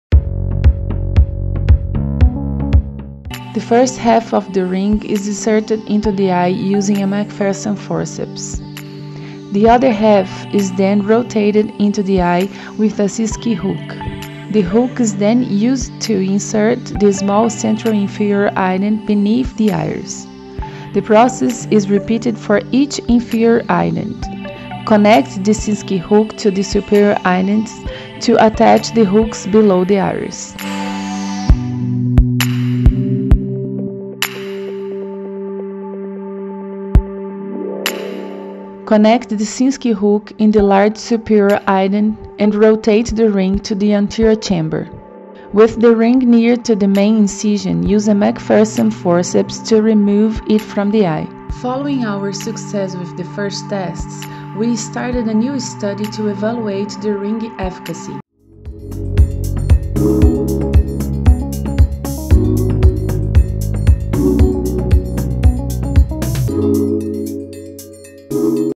CANABRAVA RING
Anillo extensor de iris diseñado para extender de forma segura el iris, facilitando las cirugías de facoemulsificación y aquellas en las que la pupila no se dilata adecuadamente mediante fármacos, consiguiéndose una apertura pupilar de 6,3 mm y, manteniéndose una dilatación estable durante la cirugía, que permite mejorar el acceso y el campo de visión del cirujano.

Tenemos el producto que estás buscando.